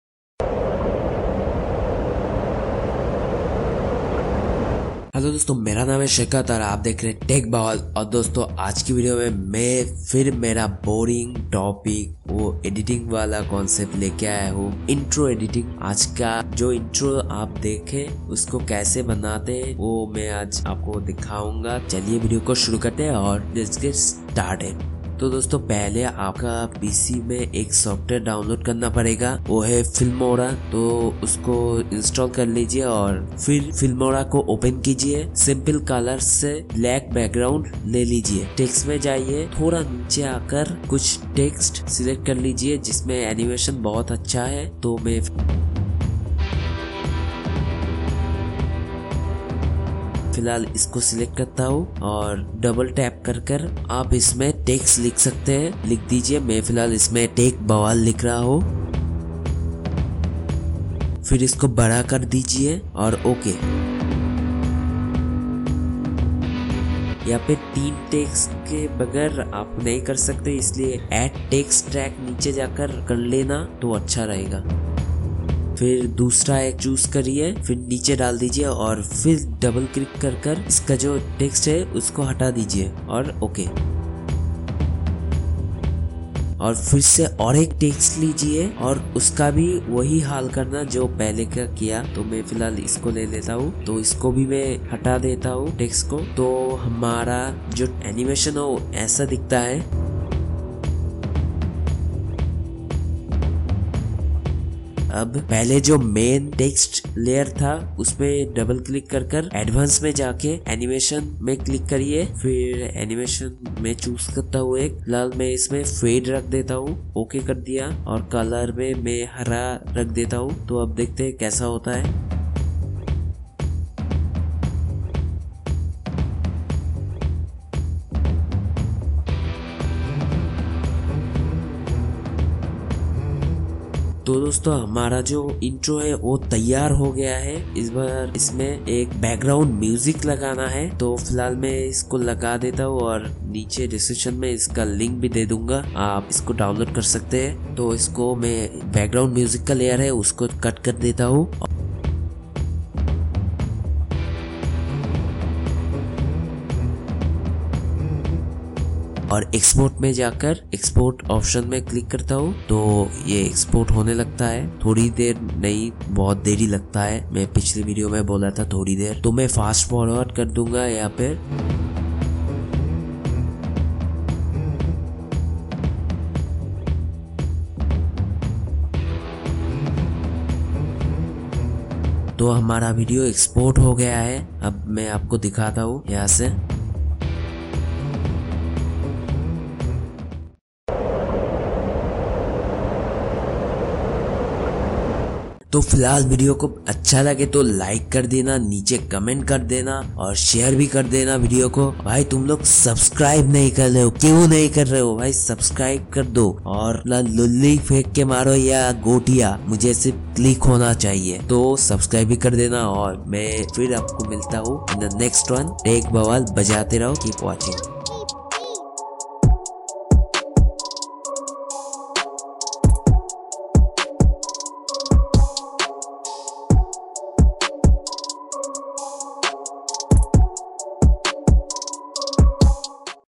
WONDERSHARE FLIMORA | HOW TO MAKE INTRO | TUTORIAL[HINDI] TECH BAWAL !
Hellow gyse mai hu upka dost or host Saikat and you are watching Tech Bawal.

To dosto ajke video mai ma upko bataunge kase ya bala intro banate hai in wondershare flimora software.

video ko dakhtai rahia ant tak or mai upse milta hu in the next one tab tak klia alvida TechBawal bajatai raho keep watching hai hind...